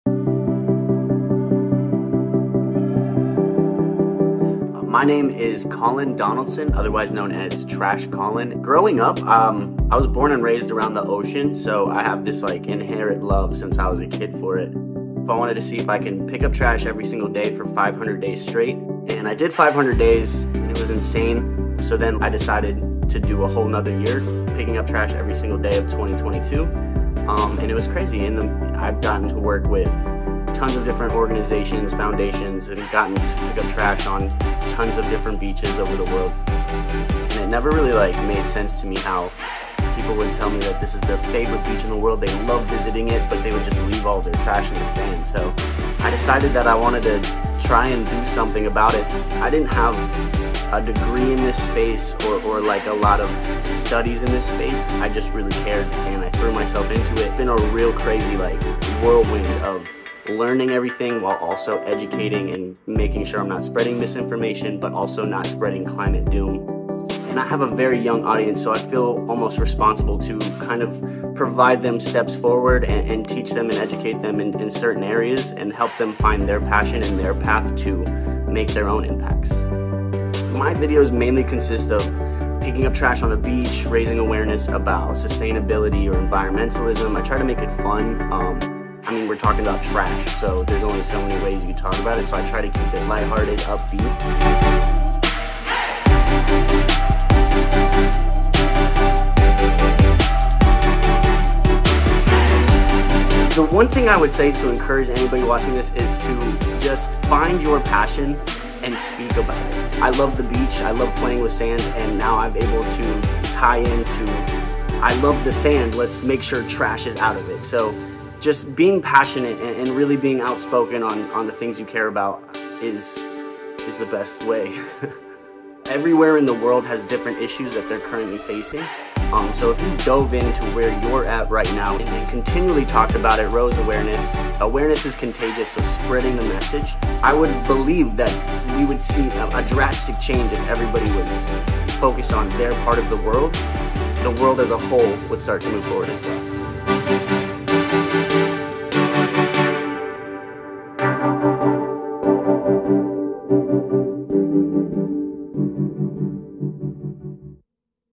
Small actions, big impact: the everyday trash picker who's making a difference
We're so inspired by the work of @TrashCaulin and his mission to make the world a cleaner, greener place! He's the perfect example of how we can have fun and still make a positive environmental impact.